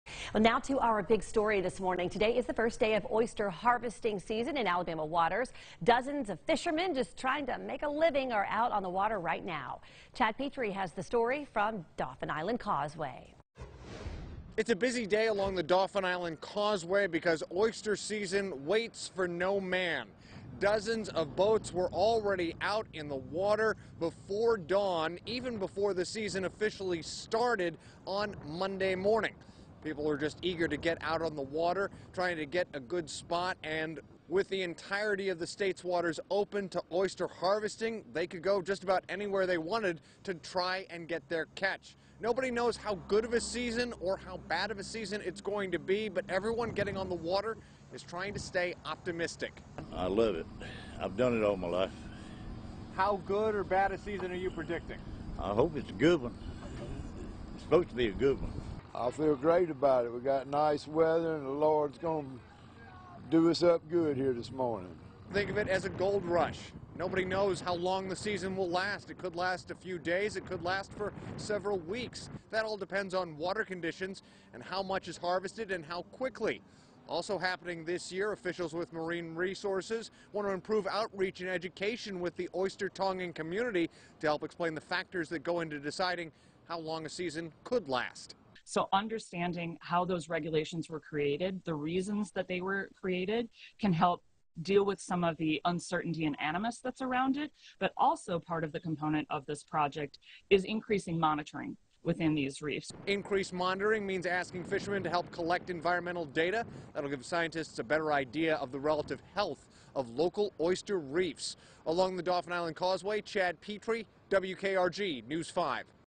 Mobile County Oystermen hoping for good season
The first day of oyster harvest season started in Alabama Monday morning